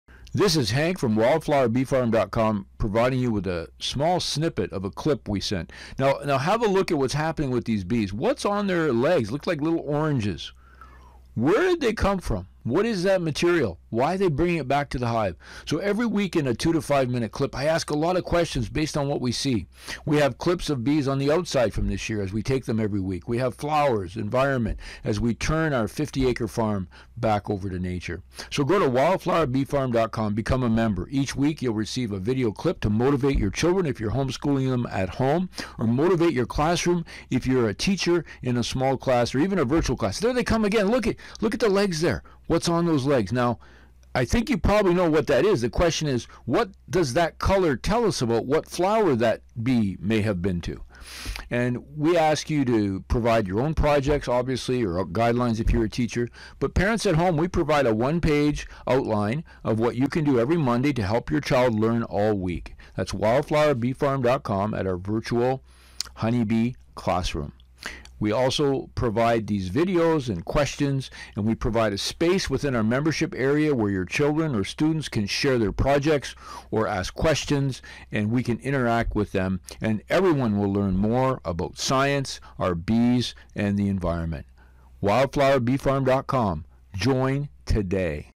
Bees With Oranges
What's on their legs?  Could it tells us where they have been?  Hank explains what the Virtual Honeybee Classroom is all about.